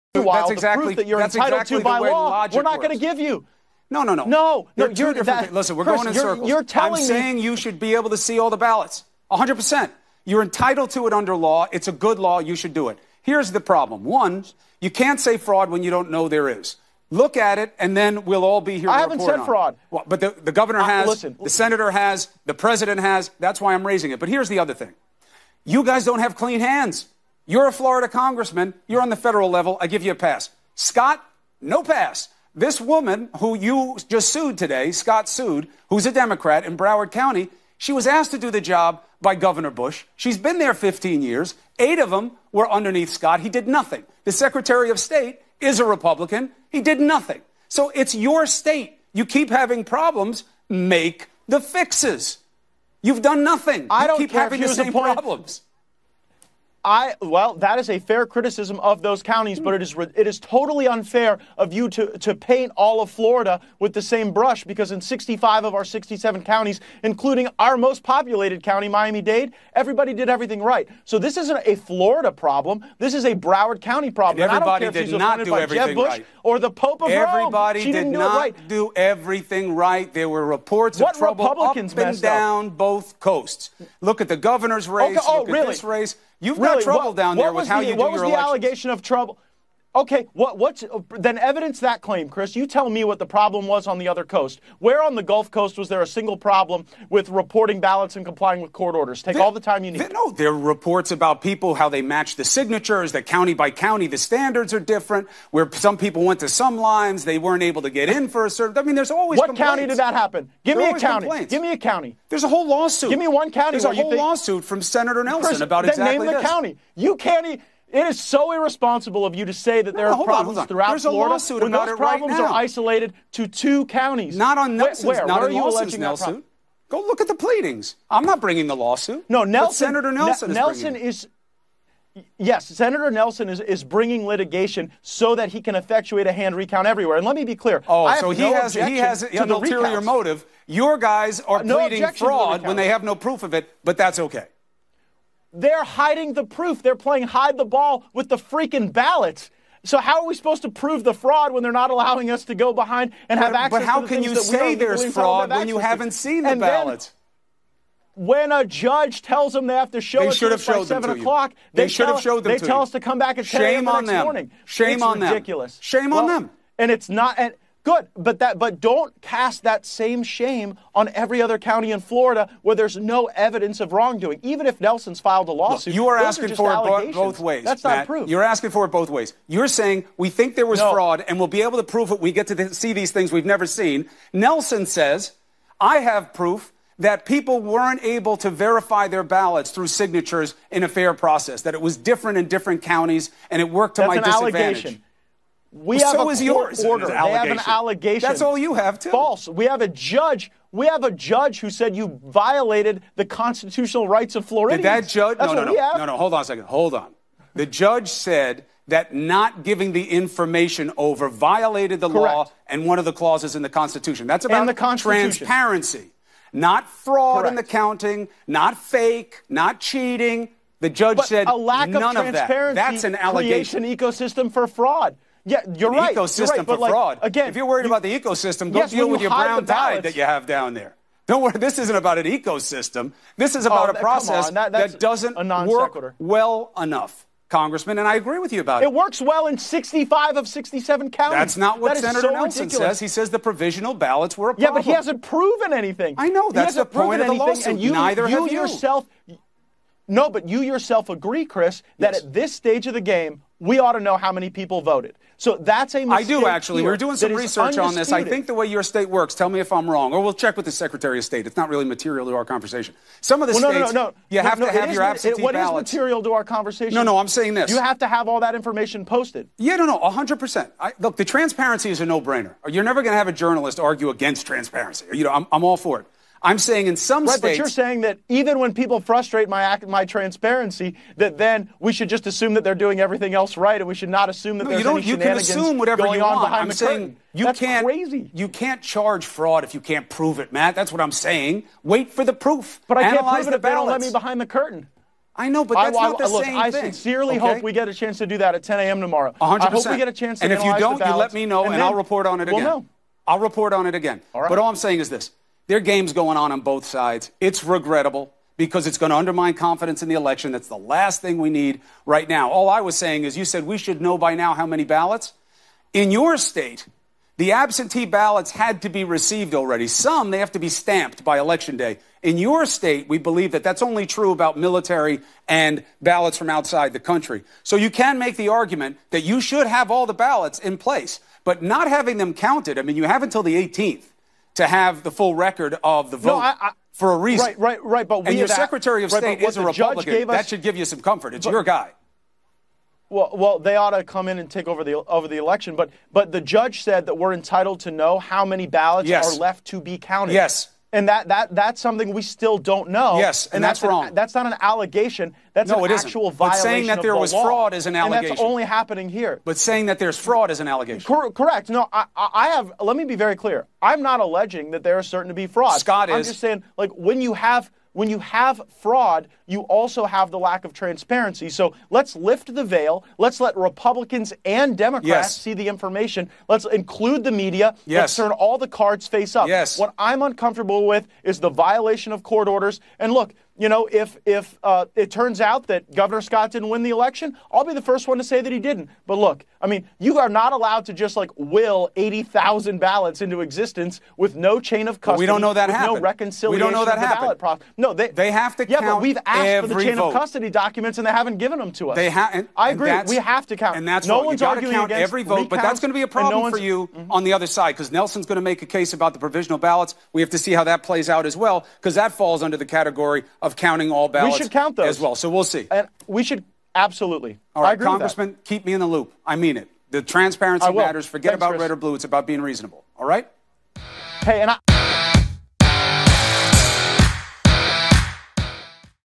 Matt Gaetz Explodes Over Florida Recount: ‘They’re Playing Hide the Ball with the Freaking Ballots!’
My channel is dedicated to bringing you a variety of truth videos that simply make you say WOW!